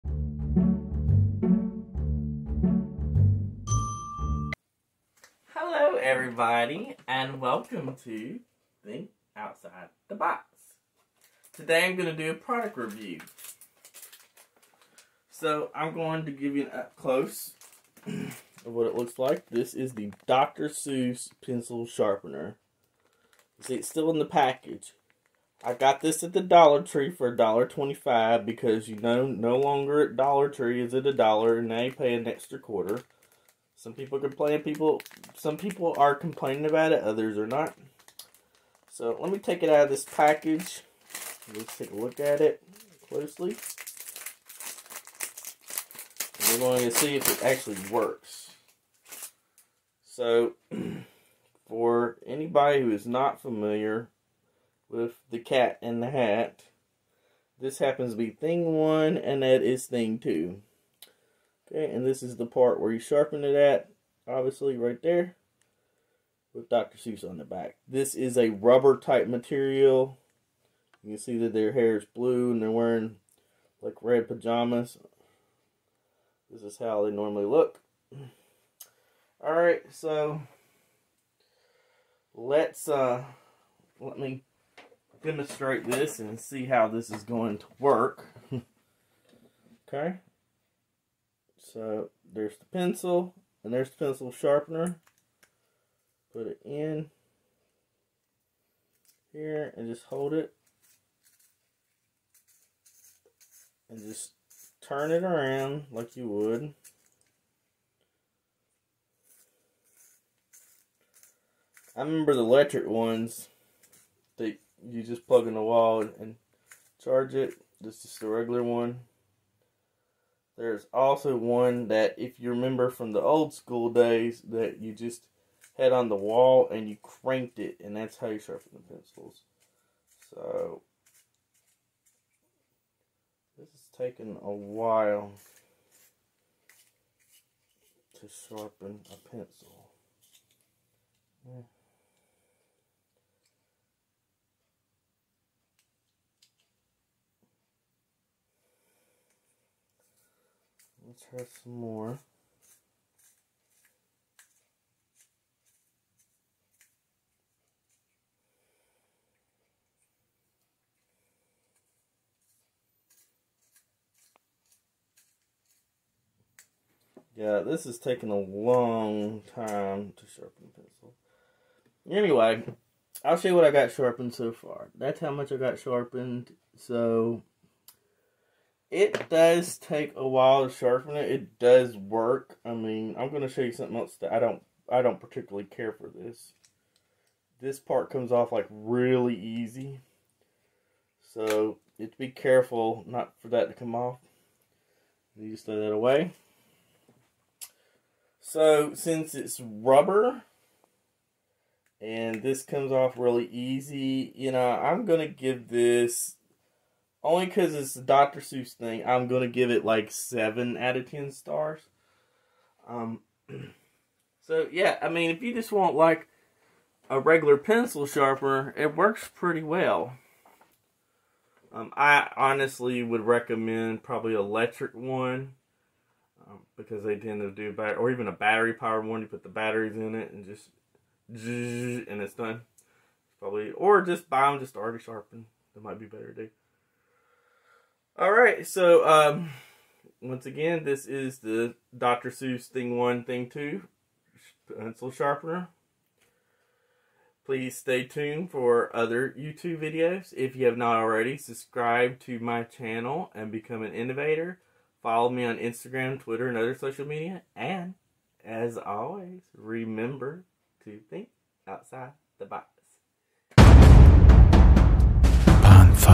DR Seuss pencil sharpener
I show you the DR Seuss pencil sharpener and give my review on it